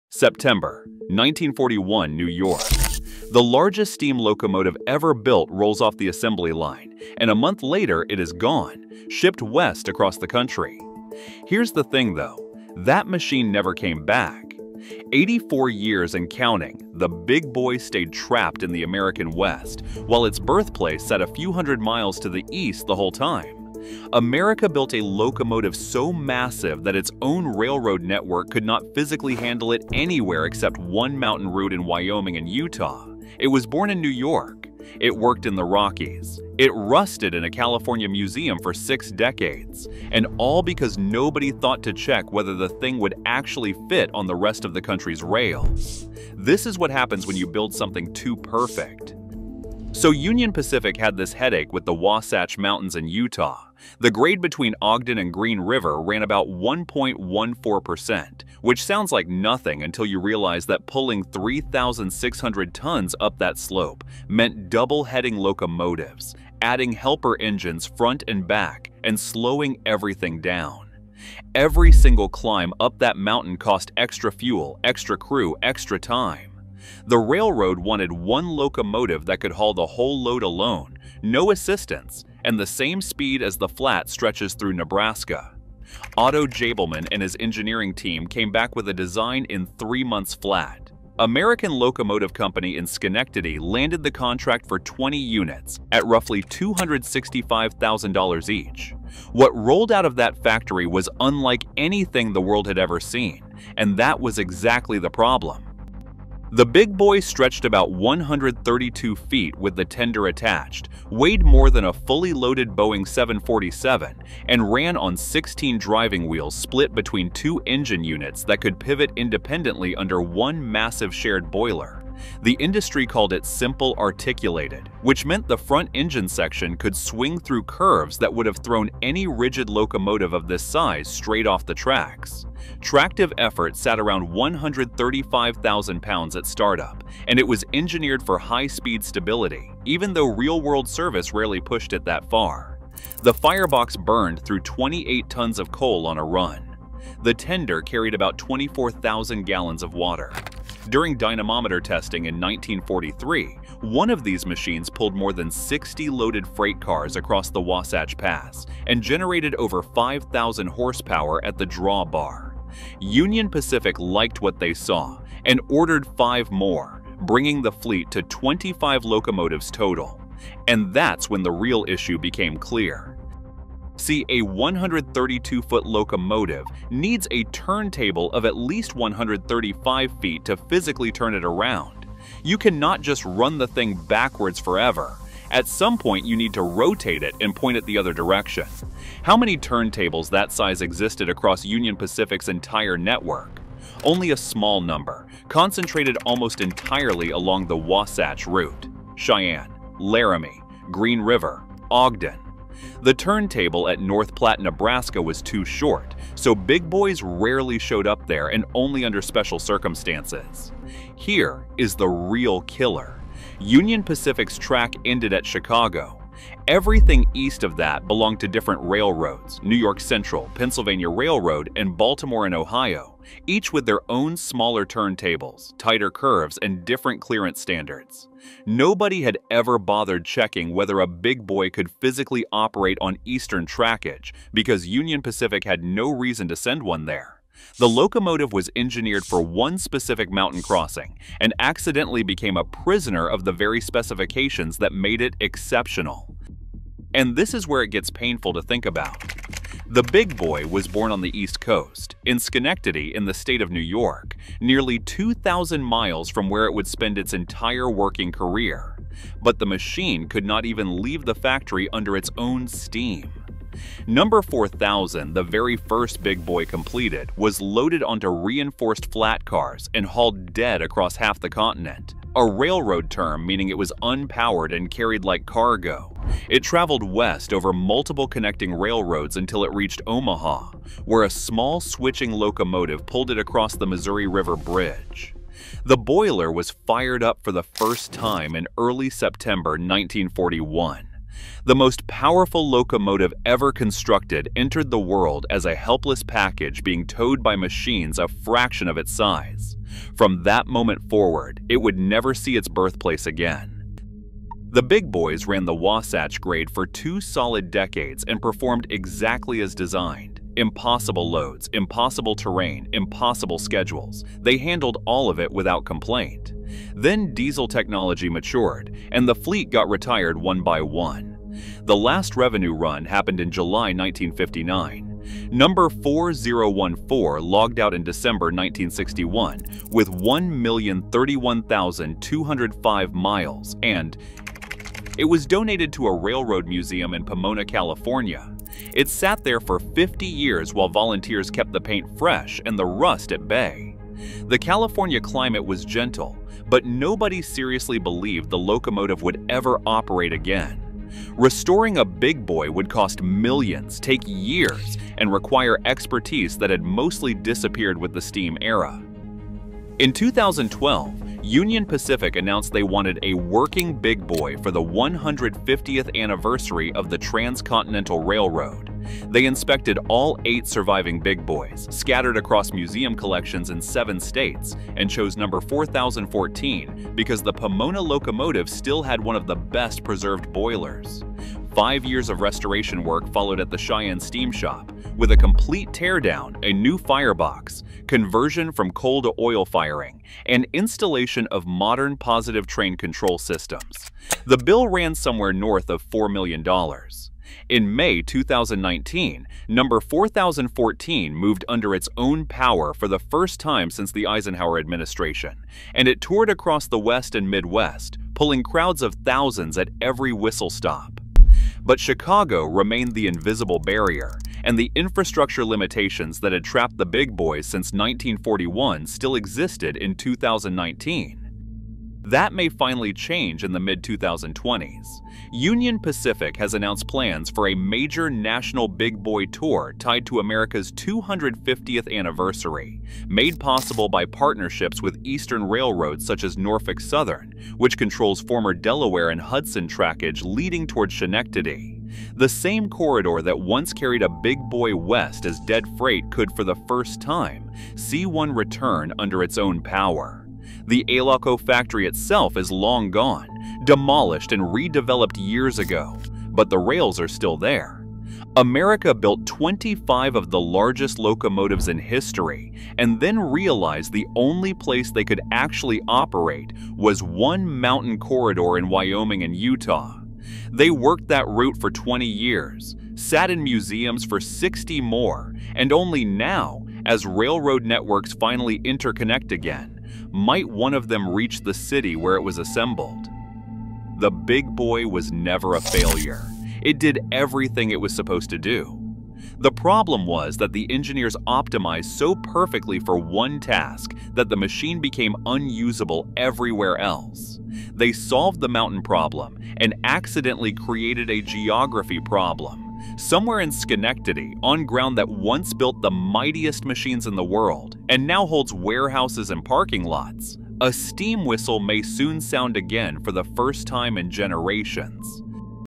Why Big Boy Was BANNED From The East Coast (Until 2026)
Why Big Boy Was BANNED From The East Coast

In September 1941, the largest steam locomotive ever built rolled out of a factory in Schenectady, New York — and immediately vanished from the place it was born. The Union Pacific Big Boy was shipped west as cargo and, for the next eighty-four years, never returned under its own power. Not because of politics or conspiracy, but because the machine was so large, so powerful, and so precisely engineered that much of America’s own rail network physically could not handle it.

Big Boy was built for one job: hauling enormous freight trains over the steep Wasatch Mountains between Ogden, Utah and Green River, Wyoming. Union Pacific needed a single locomotive that could replace multiple engines, saving time, crews, and money on one of the most expensive mountain crossings in the country. What engineers created was a monster — 132 feet long, weighing more than a loaded jetliner, with sixteen driving wheels and more than 5,000 horsepower at the drawbar.

But that perfection came at a price. A locomotive this long required enormous turntables to turn around, and only a handful existed anywhere on Union Pacific’s network — all of them located on the exact route Big Boy was designed to run. East of Chicago, where other railroads owned the tracks, the curves were tighter, the clearances smaller, and the turntables too short. No one had ever planned for Big Boy to go there, so no one built infrastructure that could support it.

The result was ironic: a locomotive born on the East Coast became trapped in the American West for its entire working life. After steam was retired, Big Boy 4014 spent decades sitting in a California museum, preserved but immobile. When Union Pacific restored it in 2019, it once again roamed the West — but Chicago remained an invisible wall.

Now, for the first time, that barrier is finally falling. Thanks to new track agreements, Big Boy is set to travel east again, retracing the same route it once traveled as a powerless shipment. After more than eight decades, America’s largest locomotive is finally going home.